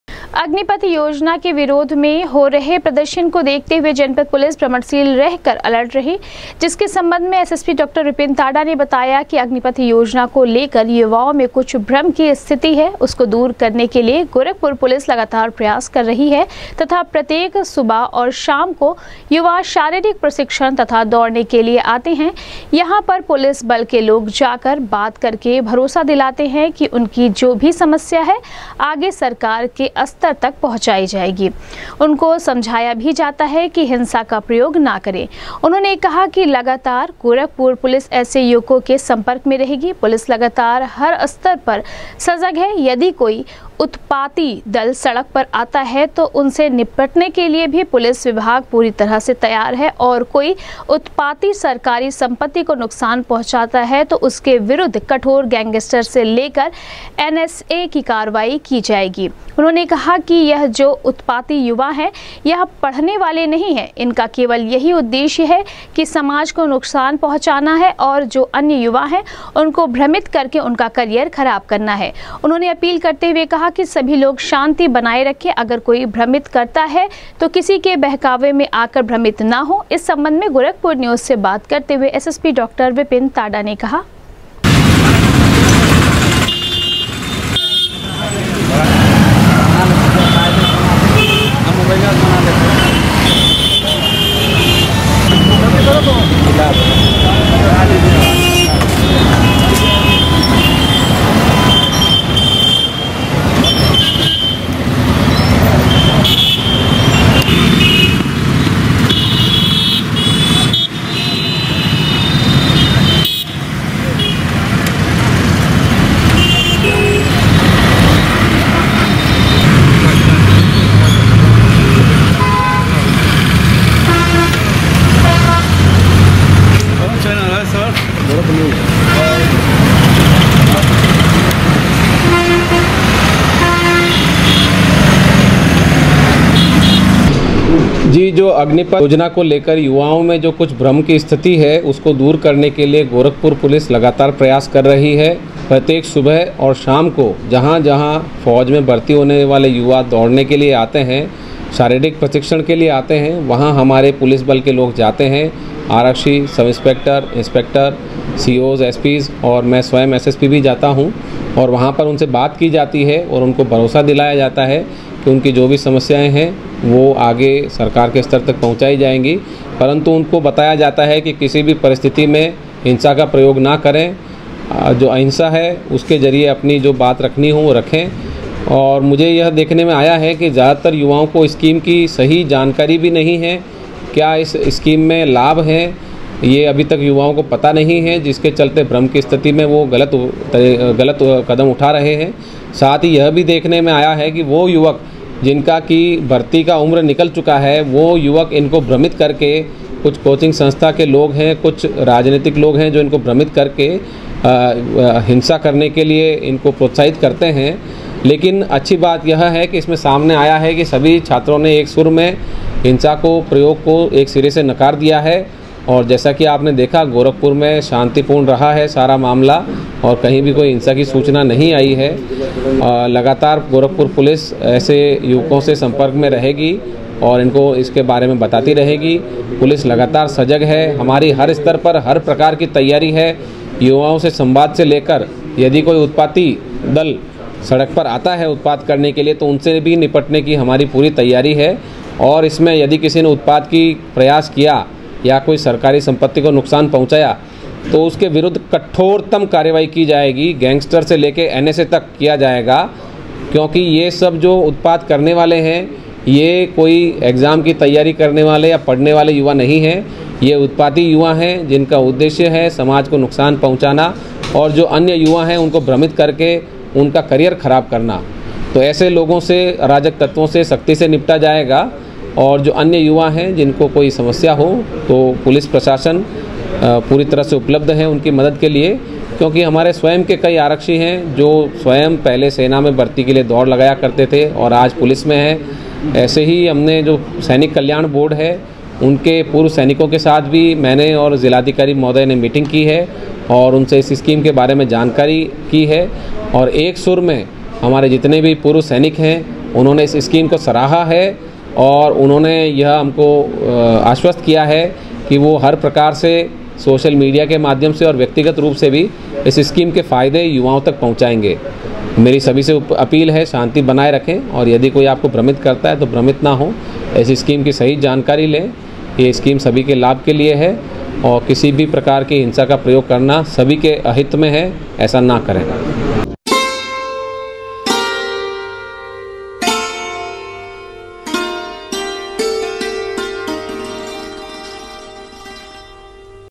अग्निपथ योजना के विरोध में हो रहे प्रदर्शन को देखते हुए जनपद पुलिस भ्रमणसील रह कर, रही अलर्ट
अग्निपथ योजना के विरोध में हो रहे प्रदर्शन को देखते हुए जनपद पुलिस भ्रमणसील रह कर, रही अलर्ट, एसएससी डॉ विपिन ताड़ा ने कहा,अग्निपथ योजना को लेकर के युवाओं में है कुछ भ्रम की स्थिति, गोरखपुर पुलिस भ्रम दूर करने के लिए लगातार कर रही है प्रयास,किसी के बहकावे में आकर भ्रमित ना हो युवा, होगी उत्पाती दल के विरुद्ध कठोर कार्यवाही ।।